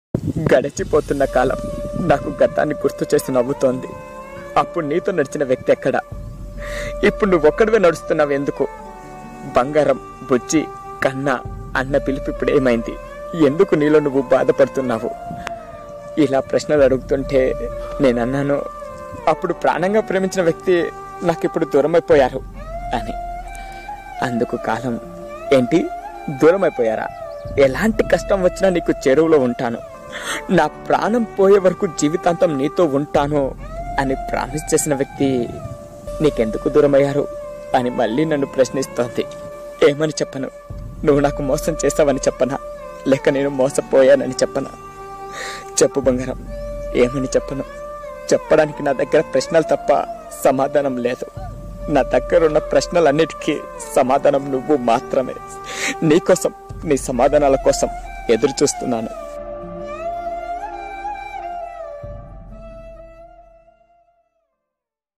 Sureshbojja Telugu Love quotes Sureshbojja Telugu Heart touching telugu prema kavithalu
గడిచిపోతున్న కాలం నాకు గతాన్ని గుర్తు చేసి నవ్వుతోంది అప్పుడు నీతో నడిచిన వ్యక్తి ఎక్కడ ఇప్పుడు నువ్వొక్కడివే నడుస్తున్నావు ఎందుకు బంగారం బుజ్జి కన్నా బేబీ అన్న పిలుపు ఇప్పుడు  ఏమైంది ఎందుకు నీలో నువ్వు బాధపడుతున్నావు ఇలా ప్రశ్నలు అడుగుతుంటే నేను అన్నాను అప్పుడు ప్రాణంగా ప్రేమించిన వ్యక్తి ఇప్పుడు నాకు దూరం అయ్యారు అని  అందుకు కాలం ఏంటి దూరమయ్యారా ఎలాంటి కష్టం వచ్చినా నీకు చేరువలో ఉంటాను నా ప్రాణం పోయే వరకు జీవితాంతం నీతో ఉంటాను అని ప్రామిస్ చేసిన వ్యక్తి నీకు ఎందుకు దూరం అయ్యారు అని మళ్ళీ ప్రశ్నిస్తోంది ఏమని చెప్పను నువ్వు నాకు మోసం చేస్తావని చెప్పనా లేక నేను మోసపోయాను అని చెప్పనా చెప్పు బంగారం ఏమని చెప్పను చెప్పడానికి నా దగ్గర ప్రశ్నలు తప్ప సమాధానం లేదు నా దగ్గర ఉన్న ప్రశ్నలన్నింటికీ సమాధానం నువ్వు మాత్రమే నీ కోసం నీ సమాధానాల కోసం ఎదురుచూస్తున్నాను